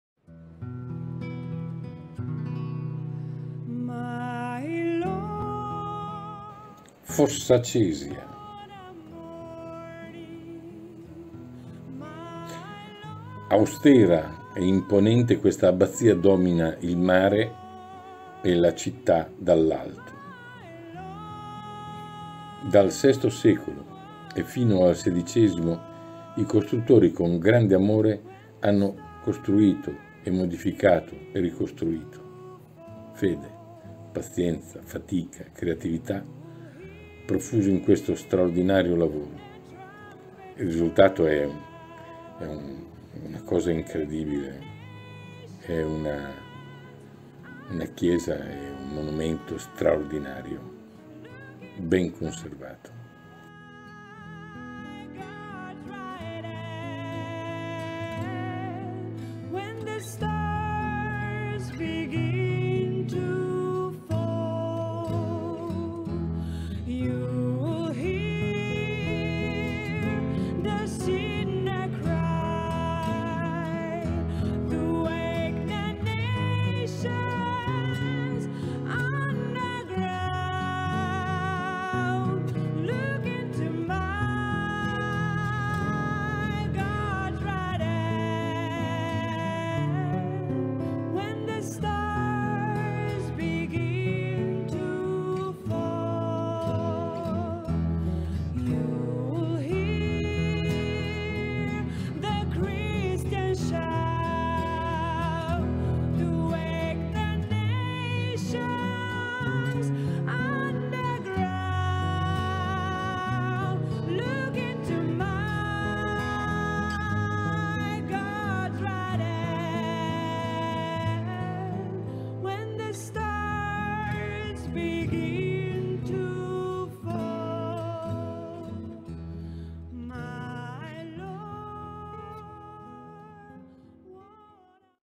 Fossacesia - San Giovanni in Venere
Austera e imponente questa abbazia domina il mare e l'abitato di Fossacesia.
Dal sesto al sedicesimo secolo è stata costruita e modificata e ricostruita con grande passione.
Fede, pazienza, fatica e creatività profusi nei secoli per regalarci questo splendore.